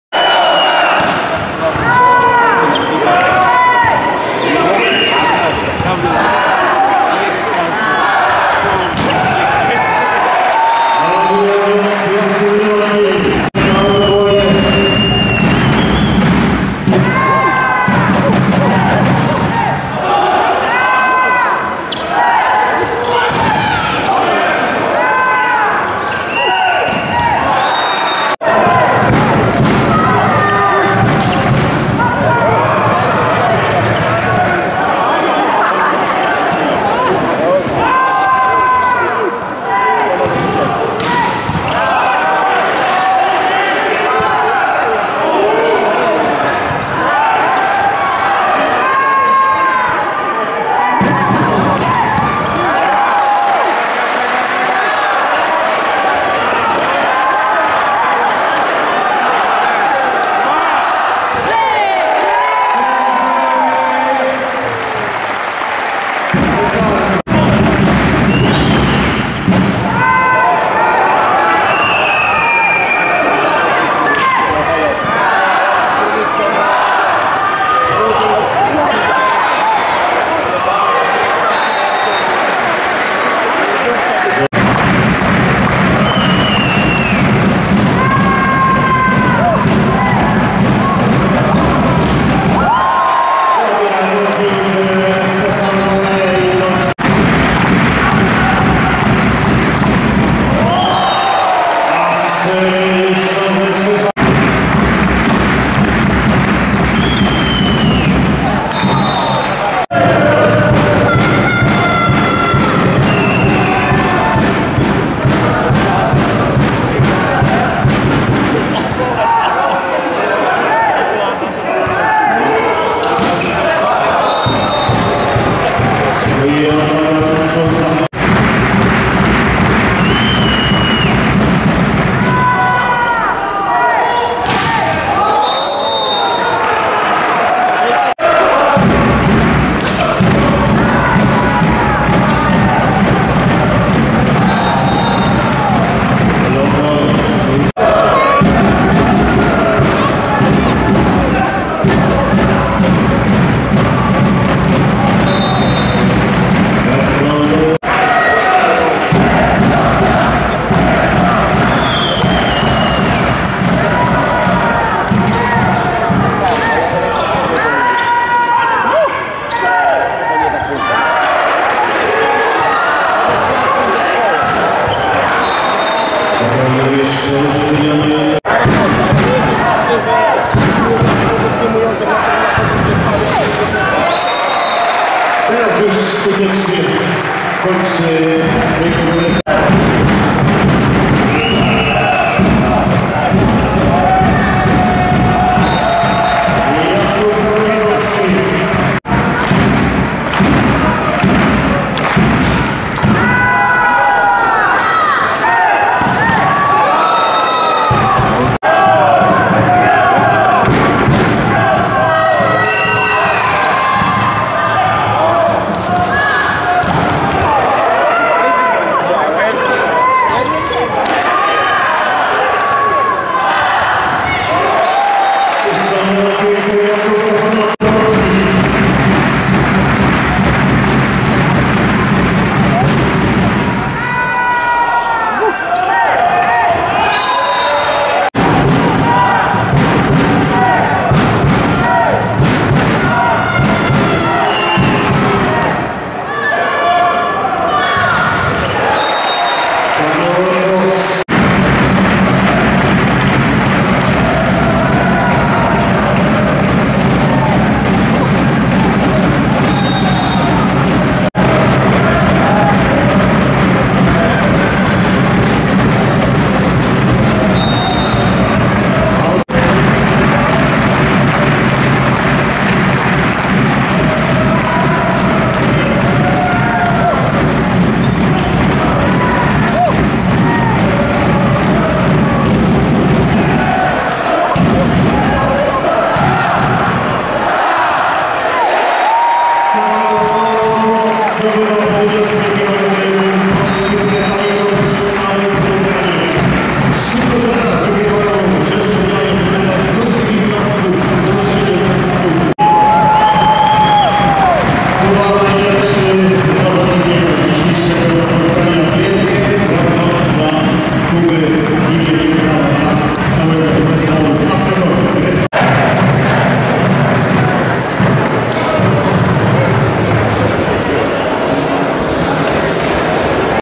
Asseco Resovia Rzeszów - Indykpol AZS Olsztyn 3:0
(25:19, 25:17, 25:20)

MVP: Jakub Kochanowski (8/10 = 80% skuteczności + 1 blok + 1 as)

Najwięcej punktów:
Rzeszów: Torey DeFalco (12), Stephen Boyer (11), Jakub Kochanowski (10) Olsztyn: Alan Souza (11), Manuel Armoa (7), Szymon Jakubiszak (6), Cezary Sapiński (6), Kamil Szymendera (6)

7. kolejka PlusLigi - hala na Podpromiu